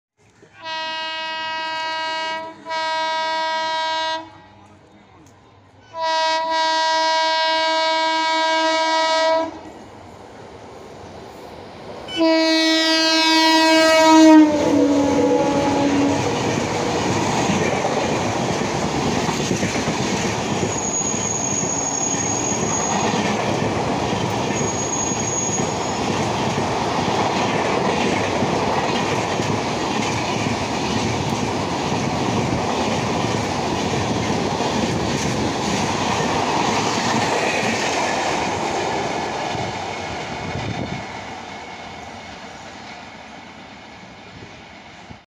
1st LHB Run Of 12703 Howrah - Secunderabad FALAKNUMA EXPRESS 👑😍 SRC WAP-7 🔥
The 1st LHB Run Of The FALCON 👑🔥 , As Train No 12703 Howrah - Secunderabad FALAKNUMA EXPRESS With Brand New LHB Coaches For The First Time Accelerating And Passing At A Decent Speed From Chengel In A Chilly Winter Morning With Santragachi (SRC) WAP-7 #39184 In Charge 🔥🔥

Clip Captured At CHENGEL ( South Eastern Railway , Howrah - Kharagpur Strech )